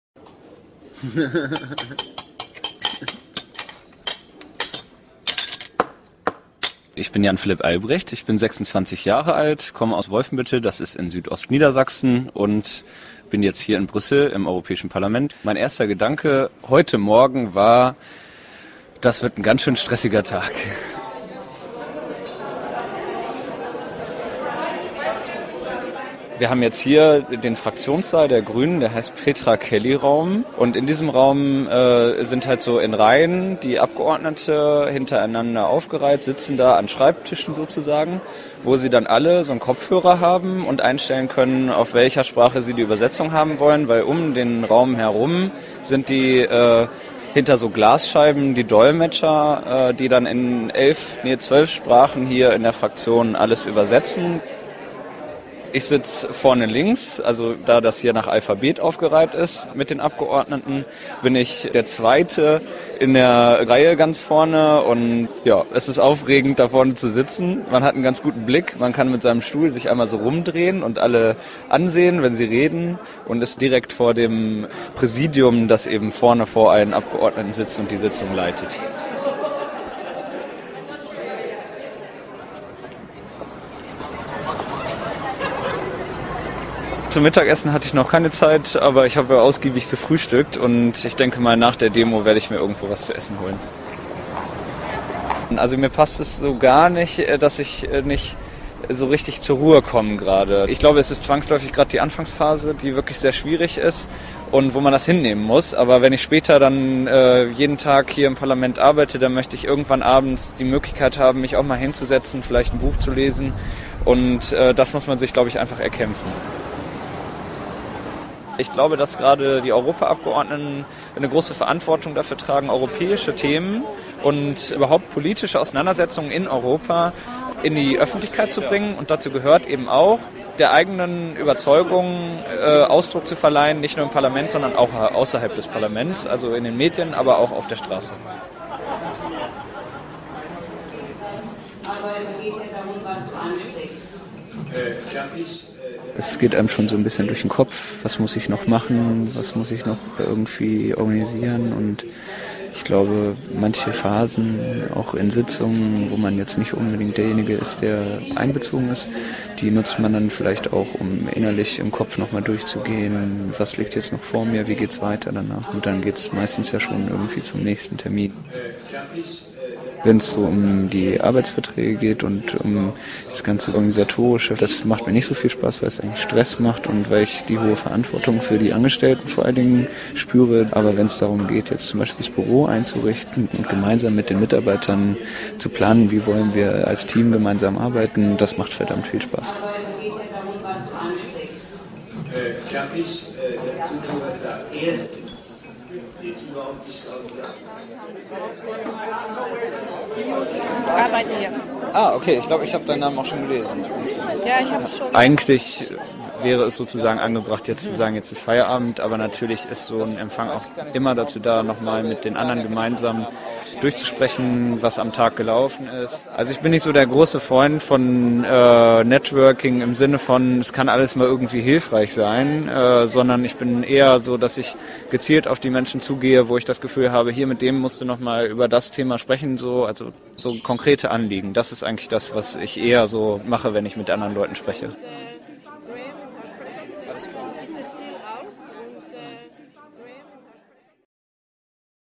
Unterwegs mit Jan Philipp Albrecht
Vom Frühstück bis zum Empfang am Abend -
Jan Philipp Albrechts erste Arbeitstage in Brüssel